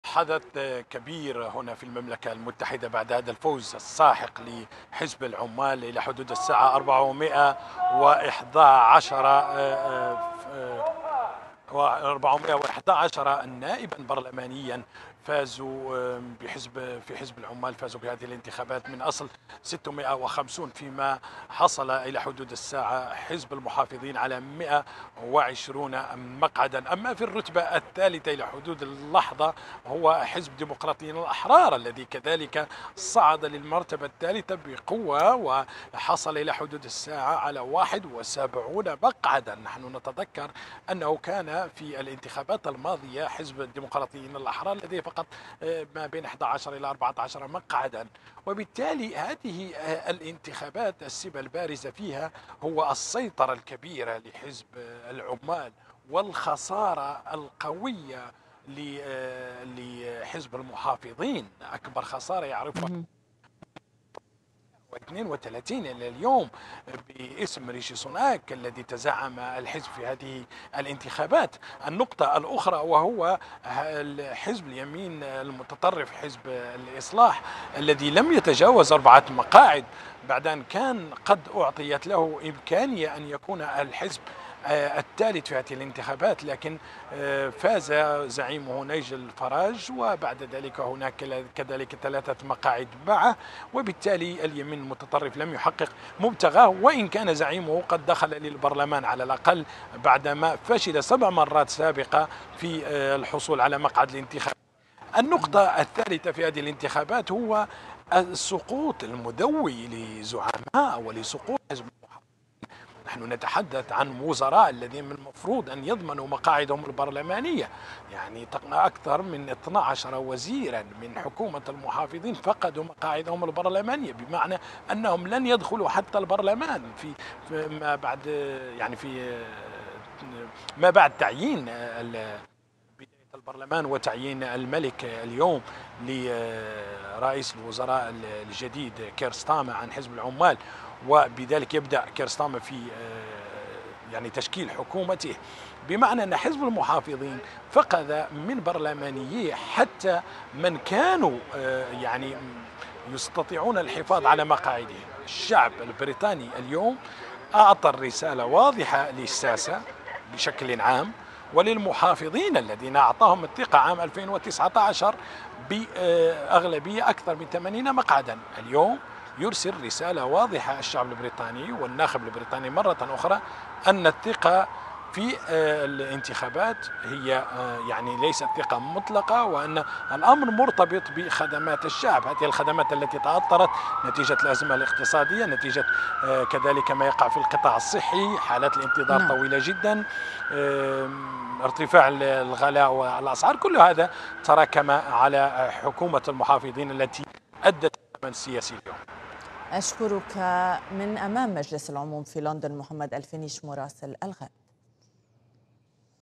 حزب العمال البريطاني يكتسح الانتخابات العامة وينهي 14 عاما من حكم المحافظين.. نتائج أولية
حزب العمال البريطاني يكتسح الانتخابات العامة وينهي 14 عاما من حكم المحافظين.. نتائج أولية مع مراسلنا محمد الفنيش في لندن